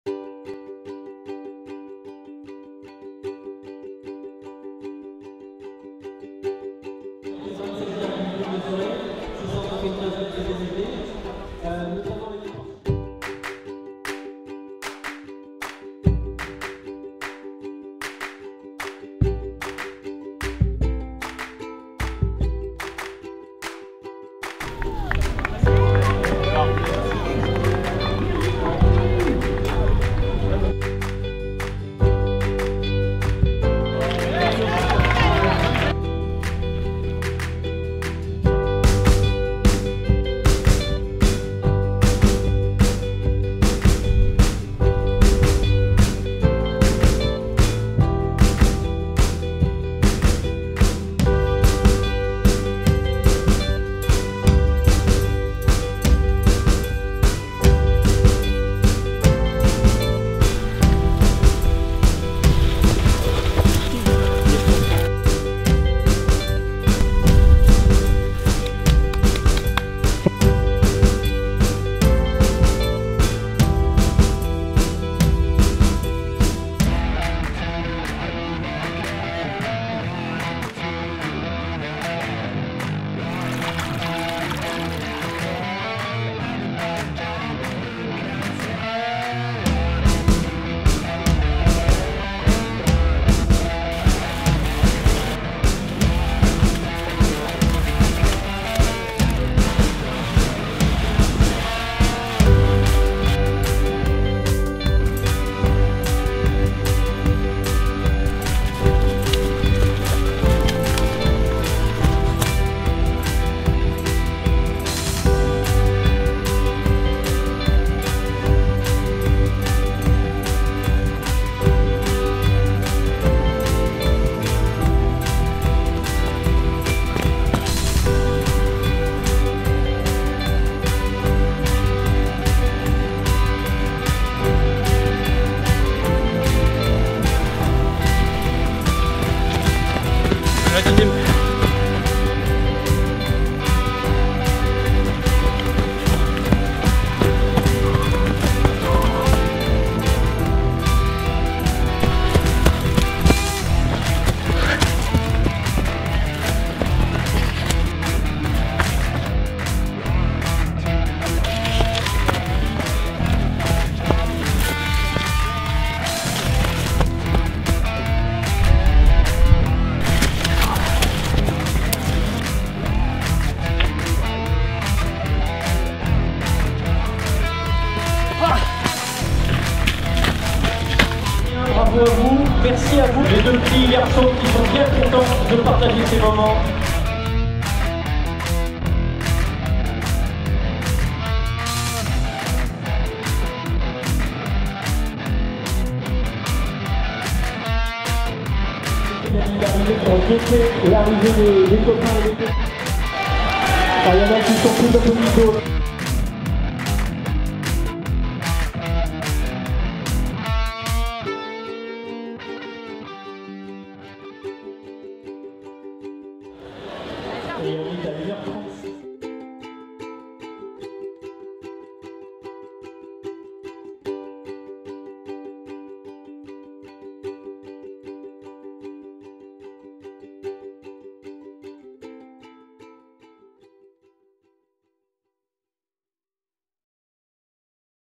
TRAIL DES SORCIERS LOYAT 2022 // Aftermovie //
Les meilleurs moments du trail des Sorciers 2022 de Loyat.
Photographe & vidéaste secteur Ploërmel